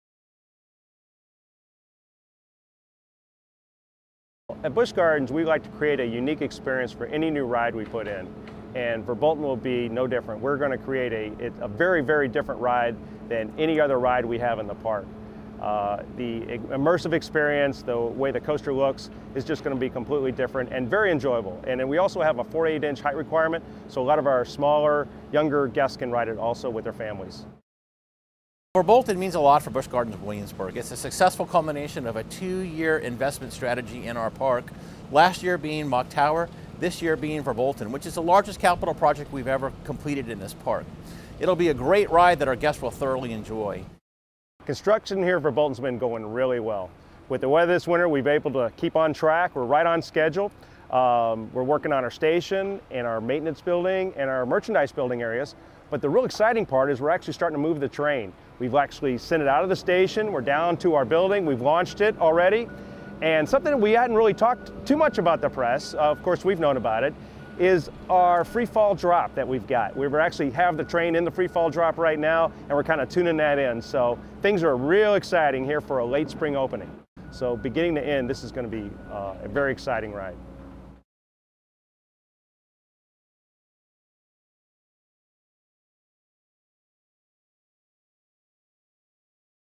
Verbolten Hard Hat Tour
We get a behind the scenes look at Verbolten, the all new coaster at Busch Gardens Williamsburg opening this Spring!